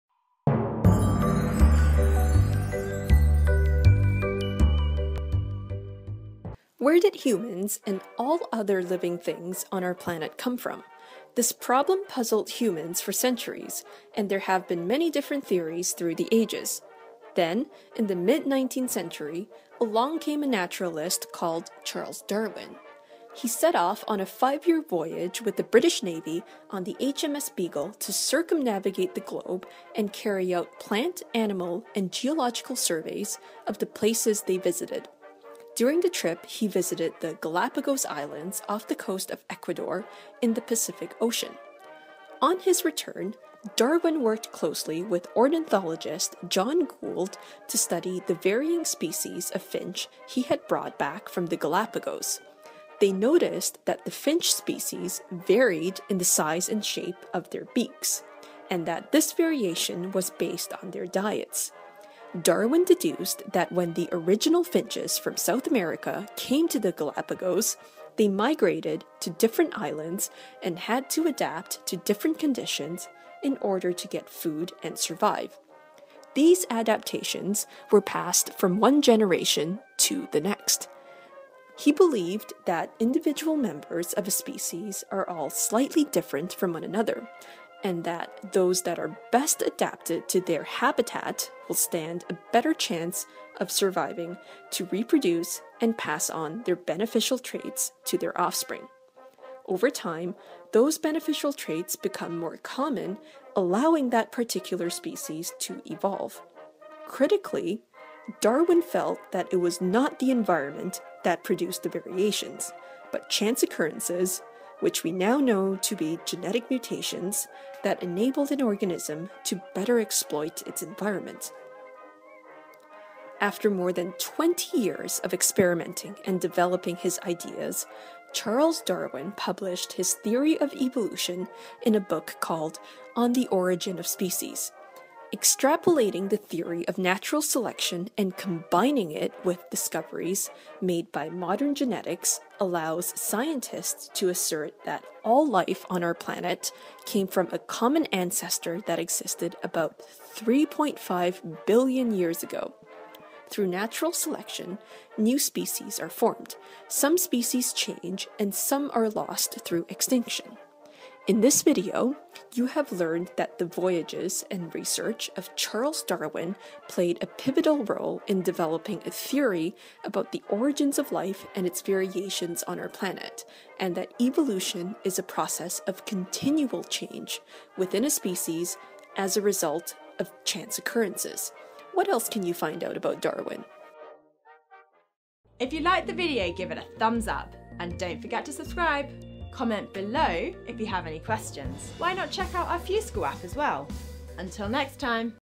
The Theory of Evolution by Natural Selection | Biology | FuseSchool
The Theory of Evolution by  Natural Selection

Where did humans, and all the other living things on our planet come from? This problem puzzled humans for centuries, and there have been many different theories through the ages. Then, in the mid 19th century, along came a naturalist called Charles Darwin. Darwin set off on a 5 year voyage with the British Navy on the HMS Beagle to circumnavigate the globe and carry out plant, animal and geological surveys of the places they visited. During the trip he visited the Galapagos Islands off the coast of Ecuador in the Pacific Ocean..

On his return, Darwin worked closely with ornithologist John Gould to study the varying species of finch he had brought back from the Galapagos. They noticed that the finch species varied in the size and shape of their beaks, and that this variation was based on their diets. Darwin deduced that when the original finches from South America came to the Galapagos, they migrated to different islands and had to adapt to different conditions in order to get food and survive. These adaptations were passed from one generation to the next.

CREDITS
Animation & Design: Waldi Apollis
Narration: Amanda Edward
Script: Amanda Edward